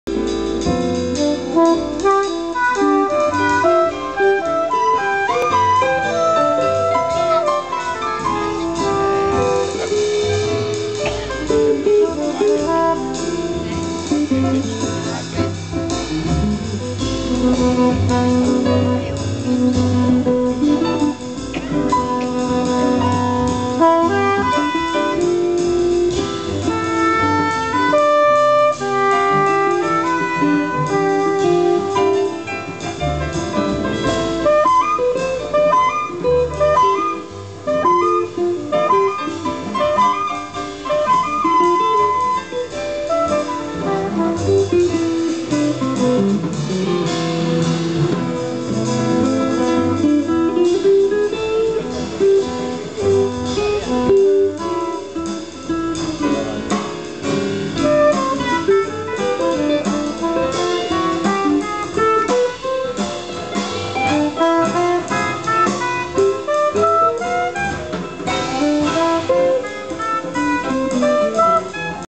Seminario Jazz a Nuoro
Concerto nel Seminario Jazz di Nuoro (direttore artistico Paolo Fresu) del chitarrista Jonh Abercrombie - al sax Tino Tracanna